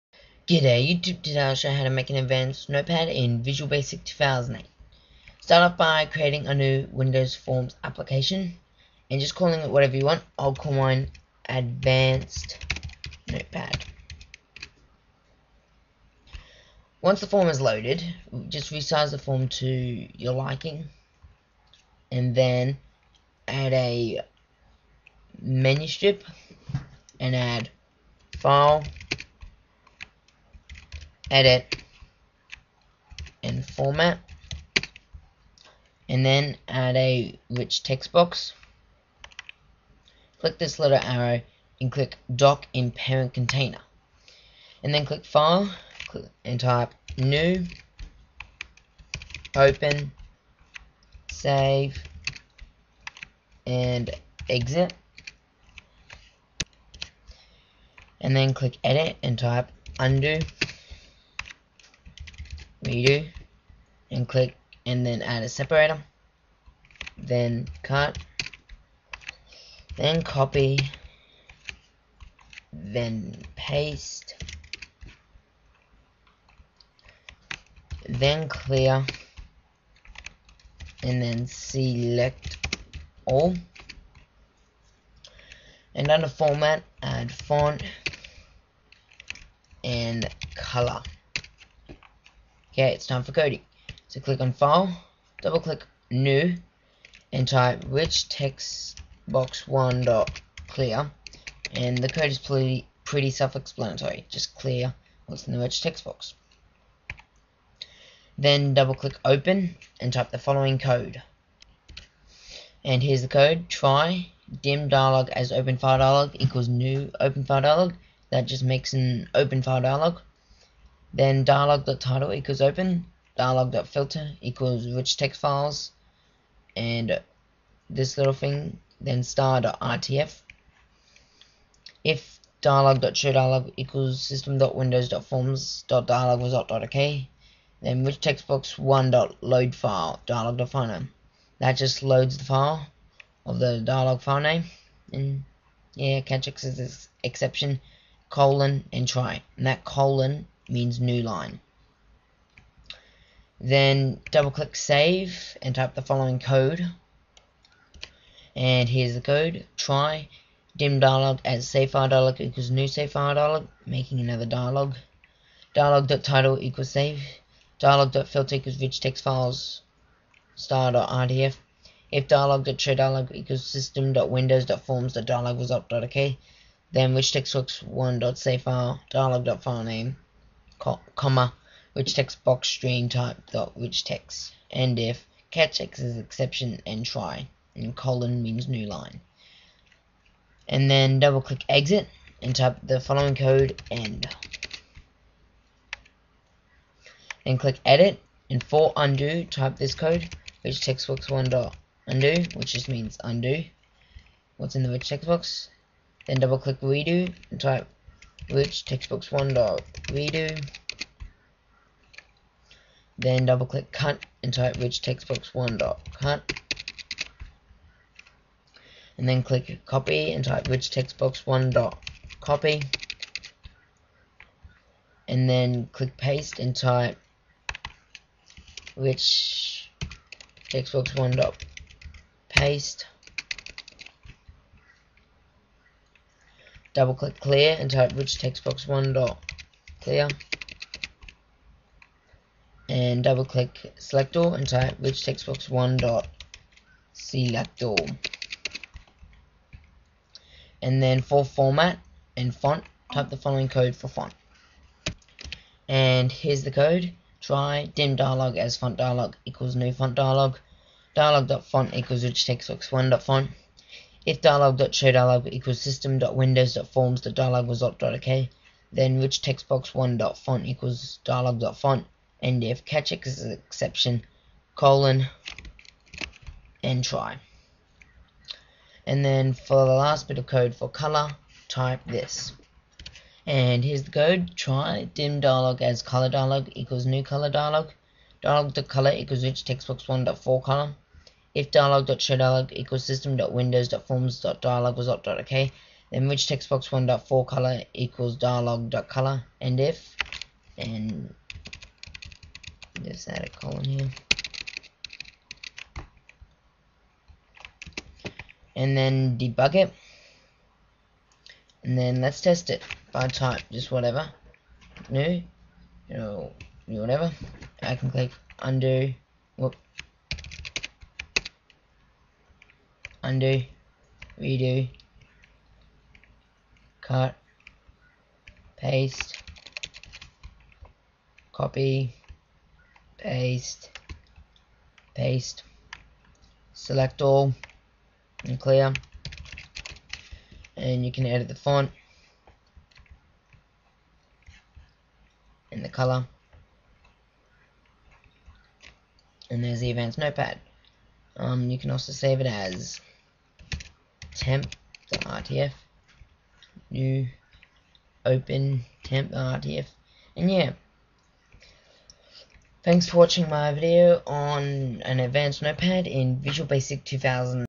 How To Make An Advanced Notepad In Visual Basic 2008
Visual Basic 2008 - Advanced Notepad. I Just remade my video on How to make a text editor in visual basic 2008 and made it it better and easier to understand. This is a tutorial on a simple Notepad which has; New, Open, Save, Font, Colour, Undo, Redo, Cut, Copy, Paste, Clear, Select All. Enjoy and here are the codes:

New:
RichTextBox1.Clear()

Open:
Try
Dim dlg As OpenFileDialog = New OpenFileDialog
dlg.Title = "Open"
dlg.Filter = "Rich Text Files (*.rtf)|*.rtf"
If dlg.ShowDialog() = System.Windows.Forms.DialogResult.OK Then
 RichTextBox1.LoadFile(dlg.FileName)
End If
Catch ex As Exception : End Try

Save:
Try
Dim dlg As SaveFileDialog = New SaveFileDialog
dlg.Title = "Save"
 RichTextBox1.SaveFile(dlg.FileName, RichTextBoxStreamType.RichText)
End If

Undo:
RichTextBox1.Undo()

Redo:
RichTextBox1.Redo()

Cut:
RichTextBox1.Cut()

Copy:
RichTextBox1.Copy()

Paste:
RichTextBox1.Paste()

Clear:
RichTextBox1.Clear()

Select All:
RichTextBox1.SelectAll()

Colour:
Try
Dim dlg As FontDialog = New FontDialog
dlg.Font = RichTextBox1.Font
 RichTextBox1.Font = dlg.Font
End If

Font:
Try
Dim dlg As ColorDialog = New ColorDialog
dlg.Color = RichTextBox1.ForeColor
 RichTextBox1.ForeColor = dlg.Color
End If